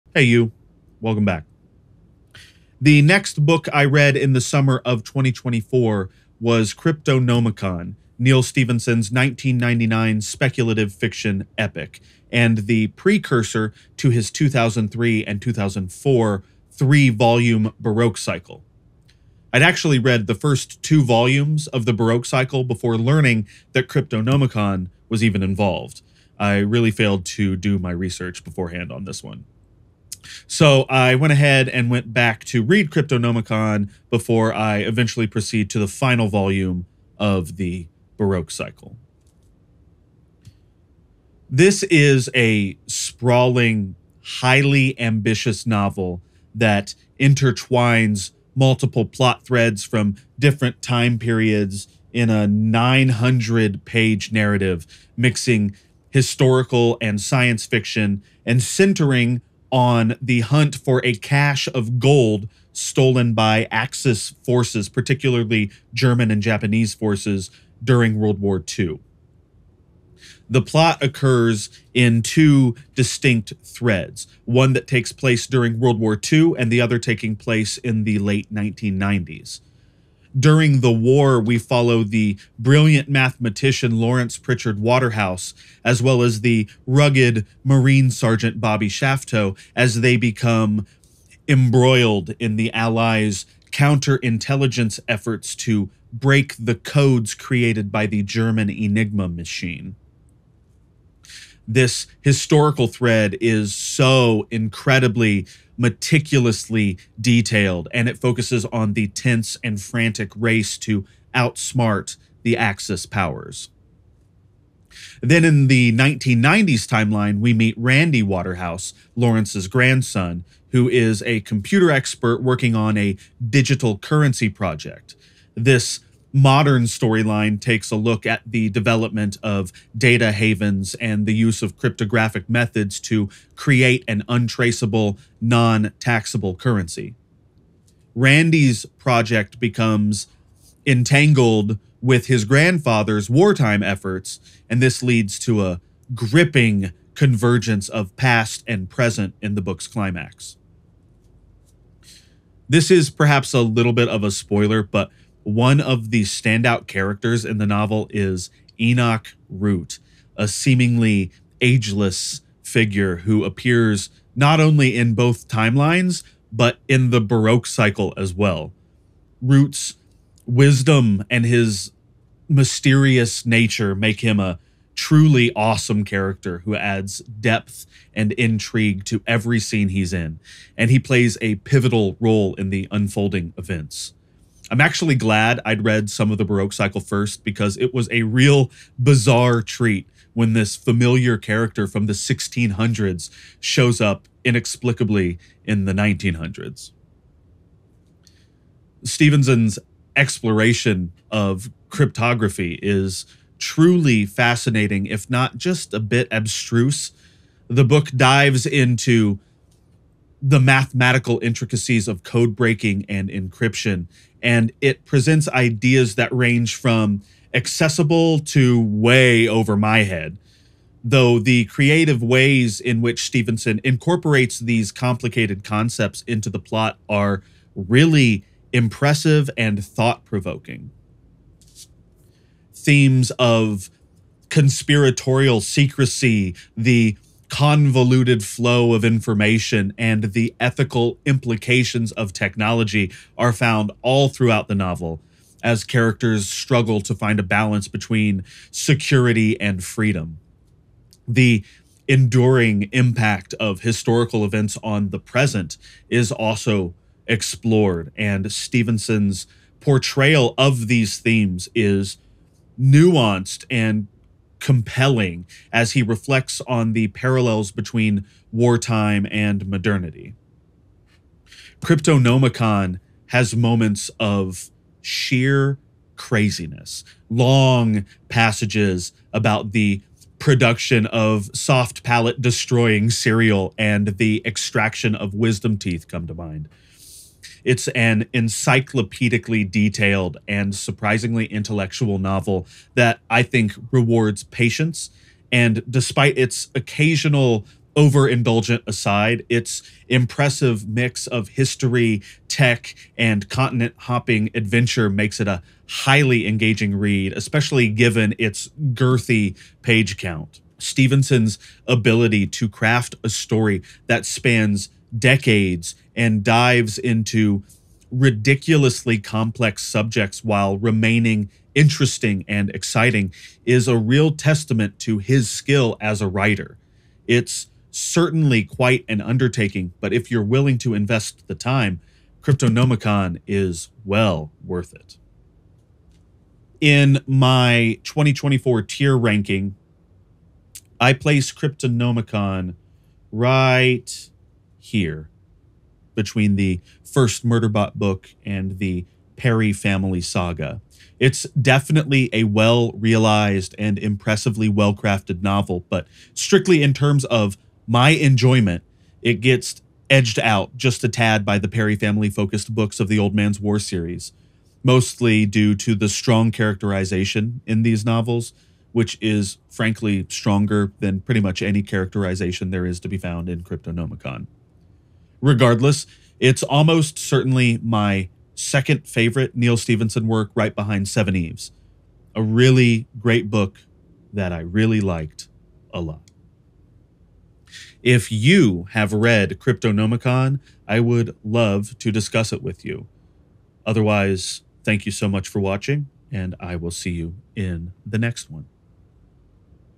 Cryptonomicon by Neal Stephenson Is Crazy
My thoughts on "Cryptonomicon" by Neal Stephenson.